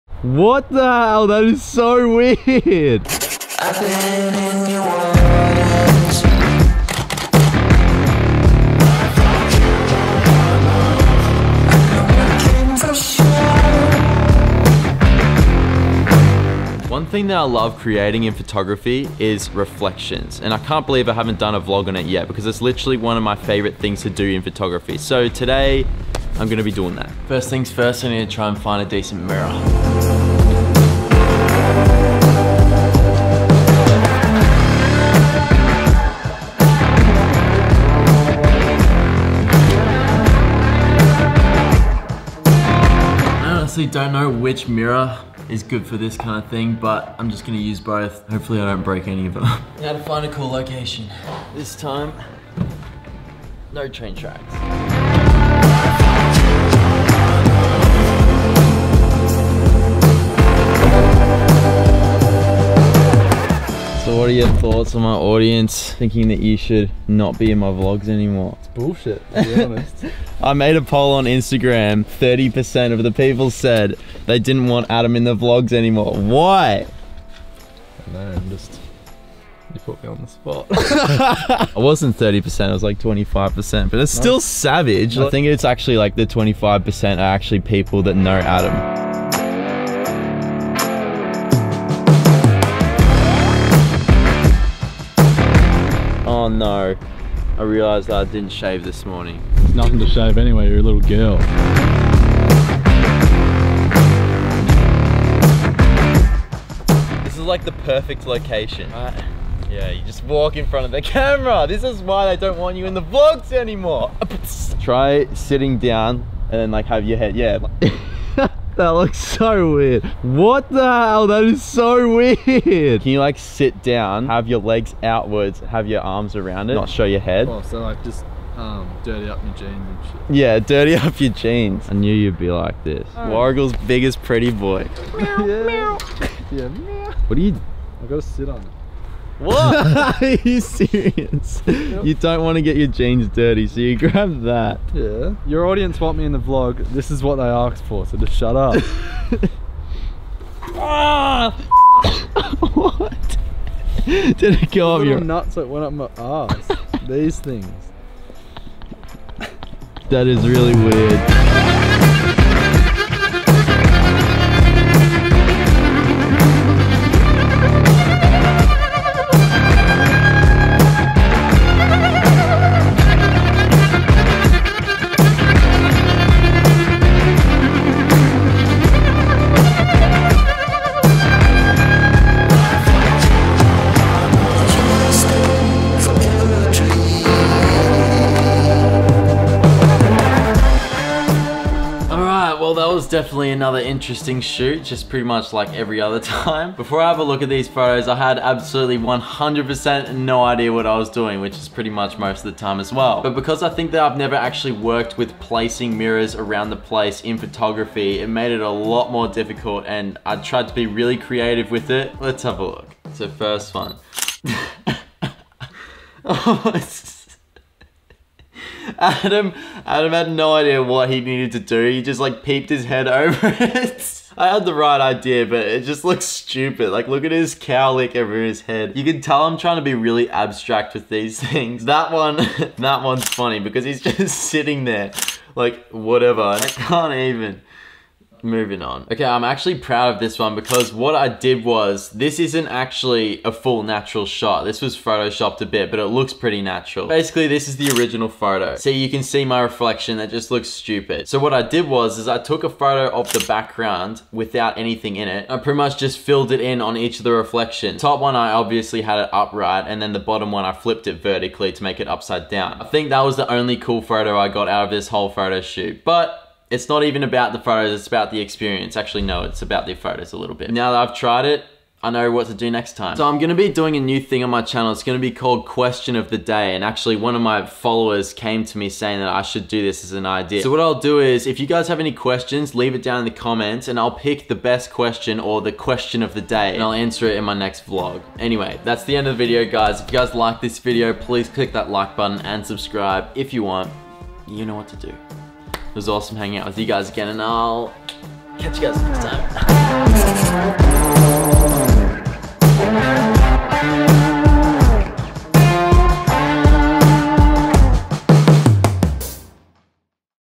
Mirror Reflection Photography is SO WEIRD
What's up guyssss! In today's vlog I attempt doing mirror reflection photography.. And it is SO weird with the illusions.. Well at least what I came up with. Also, starting a new thing on my channel called Question of the Day, where you guys ask a question in the comments and I'll answer it in my next vlog. So ask away!

If you want to support me as a creator, maybe consider checking the products on my